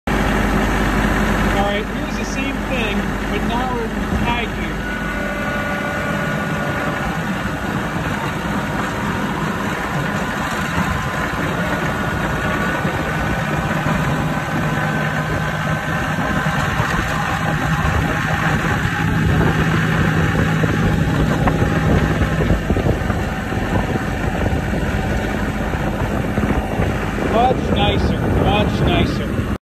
Plowing in High Gear and 4WD with 2032R
Making a swipe on the driveway this time in High Gear and 4WD. Much better. You can hear some typical hydraulic whine. But I get a much better roll of the snow off the blade. And again you see how aggressive the blade is and it is no friend of my lawn.

I will get better with the video creation :)